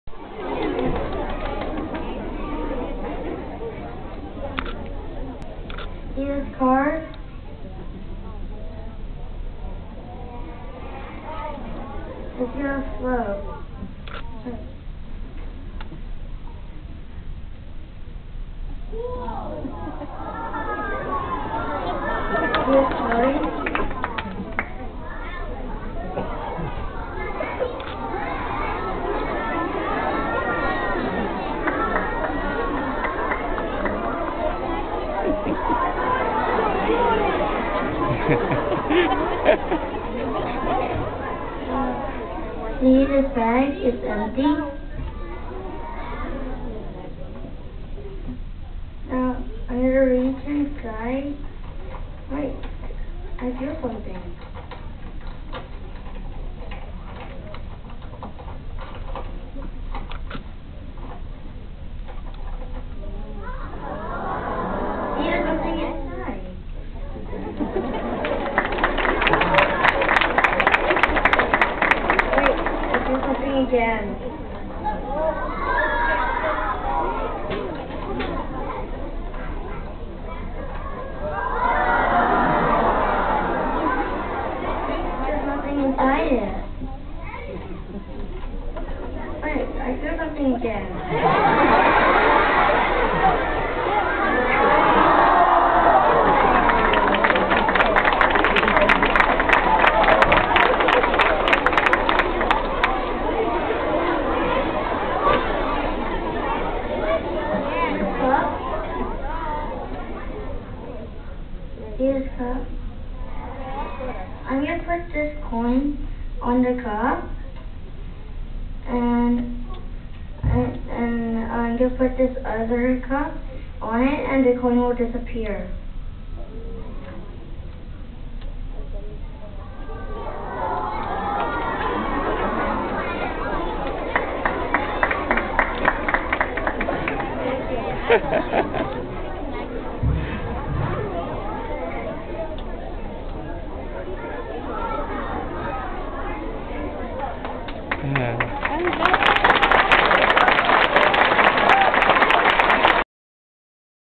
Dale Sycip the Magician
Quail Summit Elementary Talent Show: First trick - floating card, Second trick - Flowers from empty bag, Third trick - disappearing coin